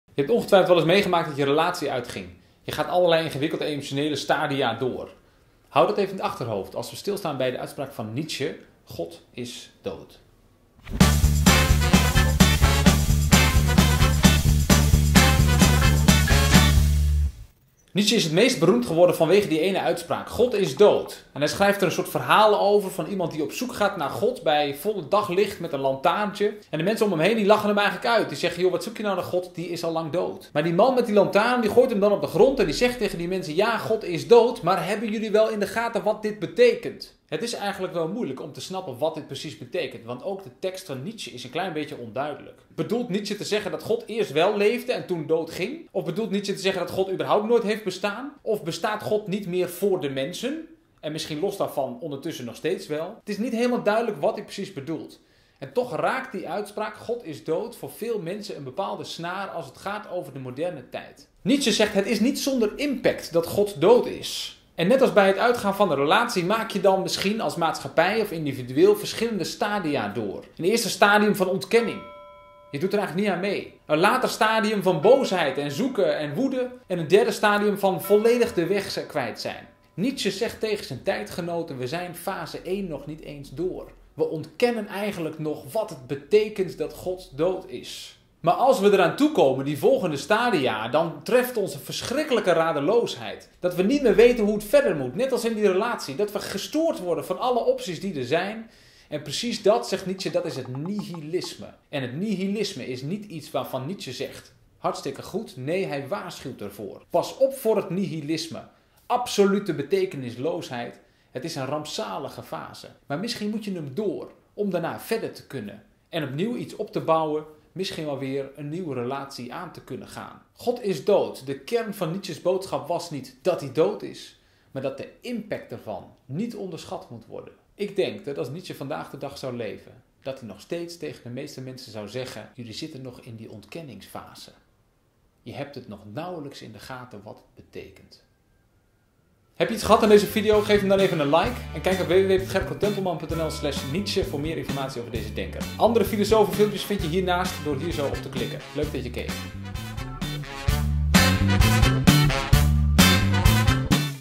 De filosoof Friedrich Nietzsche
'God is dood', zei Nietzsche. Dat weten we al. Maar wat hij verder zei, is bijna vergeten. Terwijl het 'm daar juist om ging. Gerko Tempelman legt uit hoe Nietzsche helpt in bange dagen.